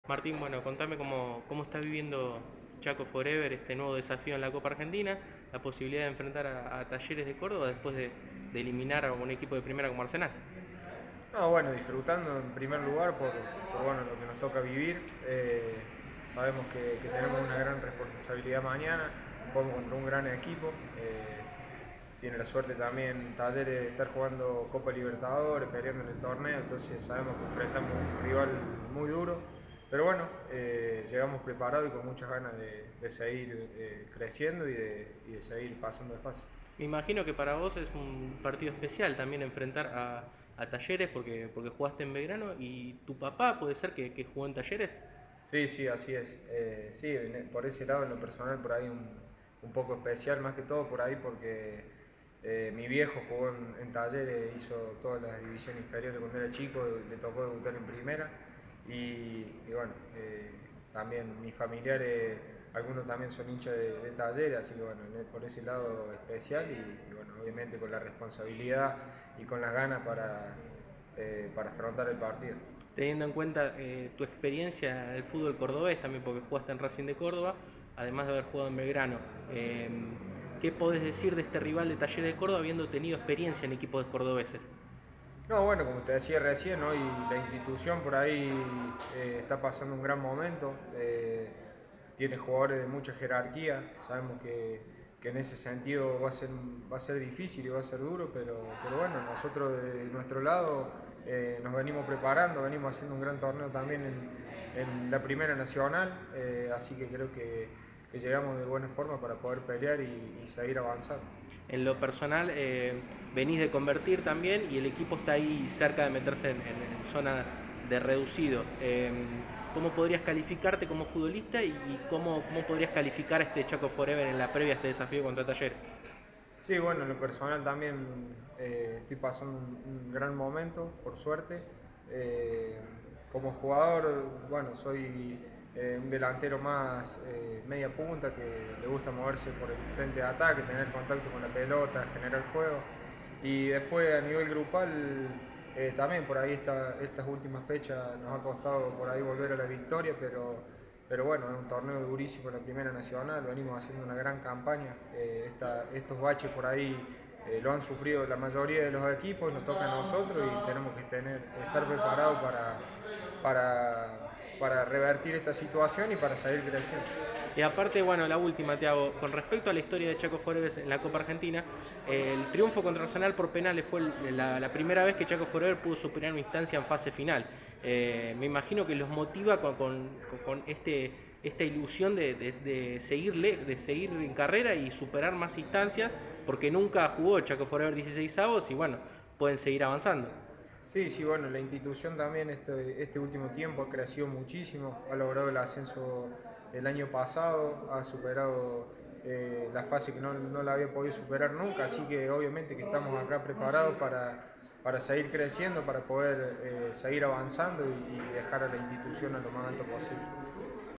Martín Garnerone - Chaco For Ever
El delantero habló con el Sitio Oficial de la Copa Argentina AXION energy, en la previa al cruce contra Talleres de Córdoba, por los 16avos de Final.